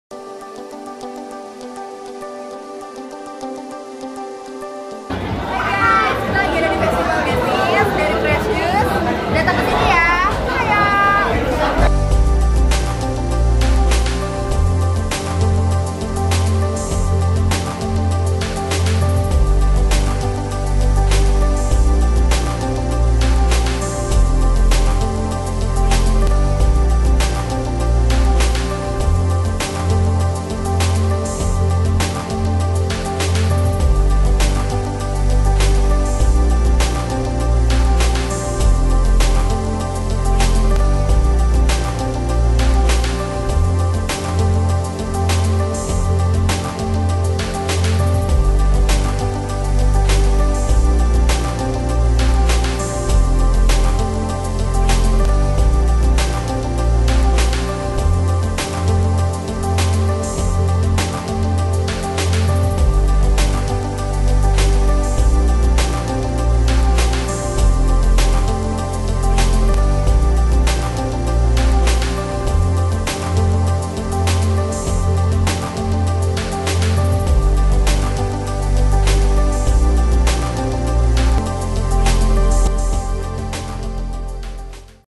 Fresh Juice | Festival Bisnis - Kewirausahaan 3 | ENFEST DESEMBER 2019
Festival Bisnis adalah salah satu rangkaian acara Entrepreneur Festival yang dilaksanakan 20-21 Desember 2019 di Universitas Mercu Buana Meruya.